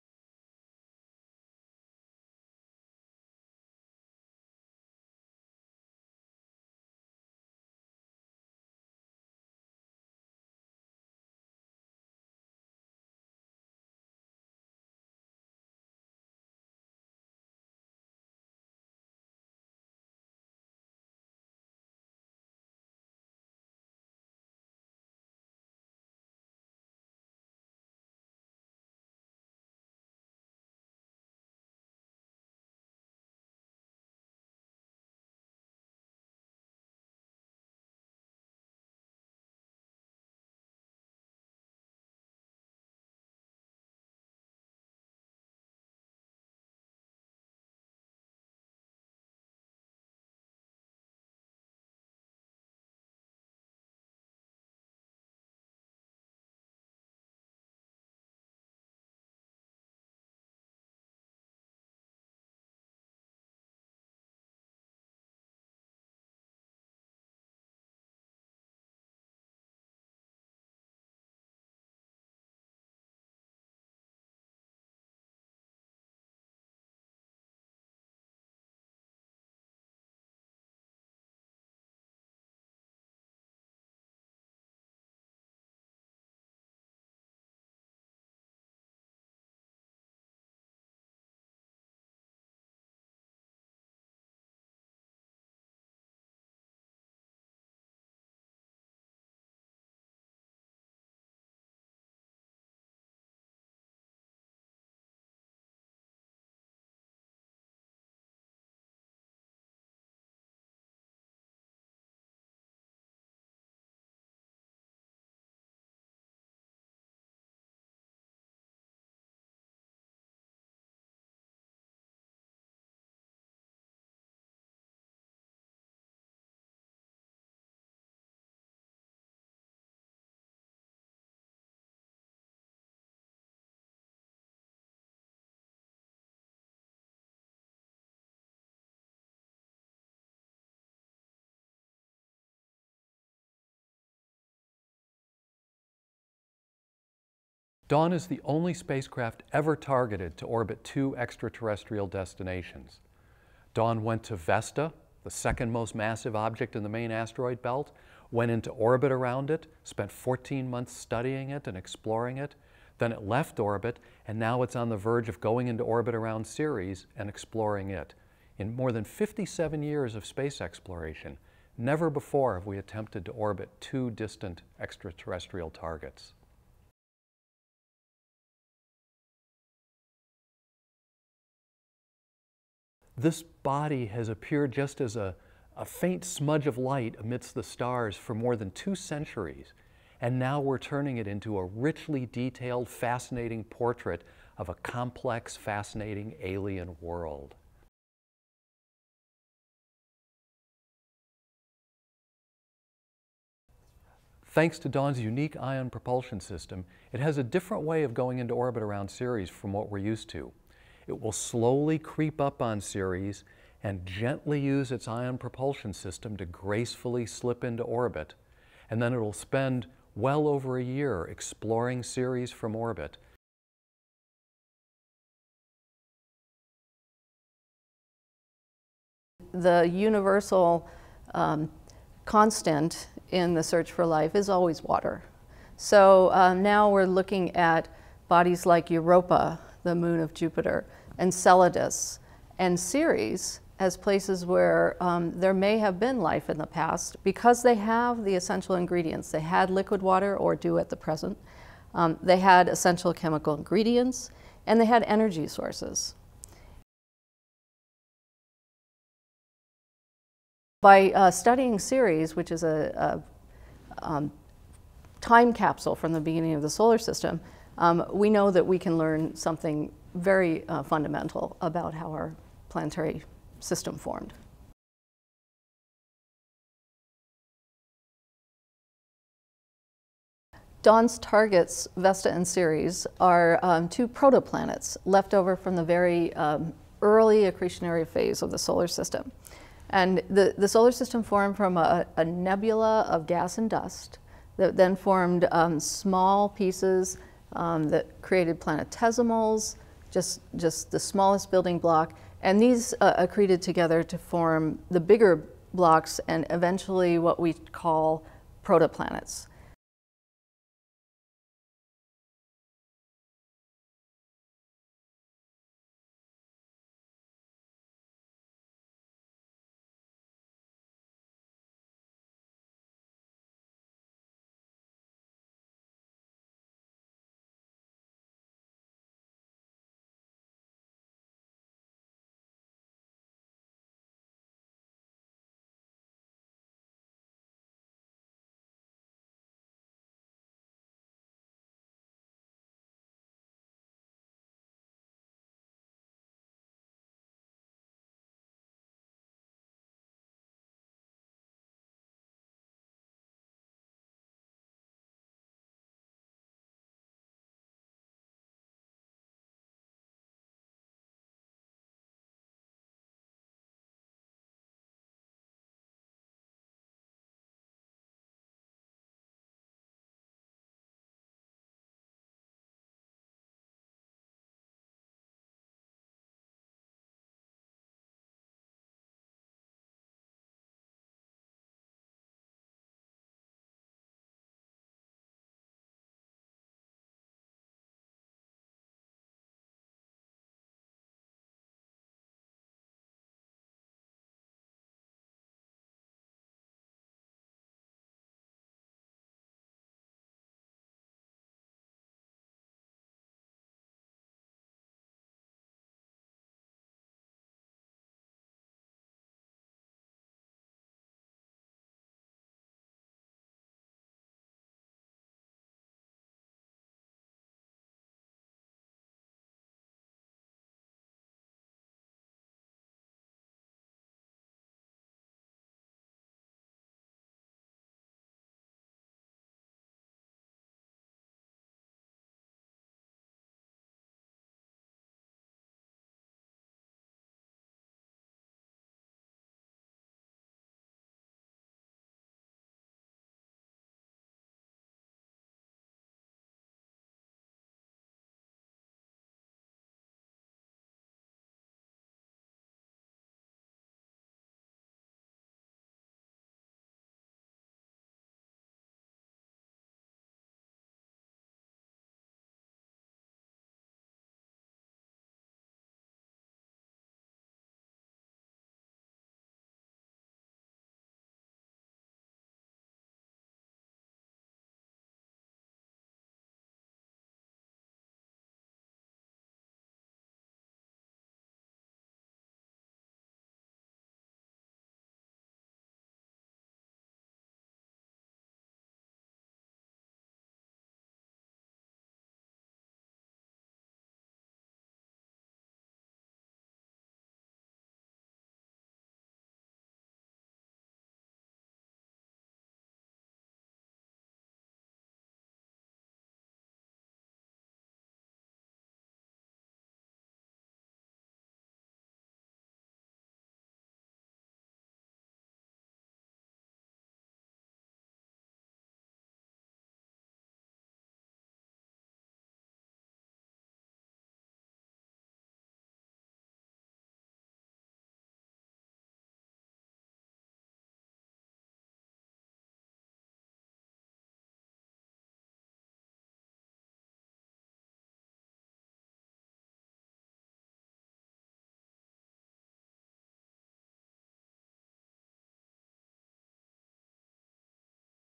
Video file: NASA's Dawn Spacecraft Ready for Ceres Orbit
Video file for media and public use.  When NASA's Dawn arrives at Ceres on March 6, 2015, it will be the first spacecraft in history to reach a dwarf planet.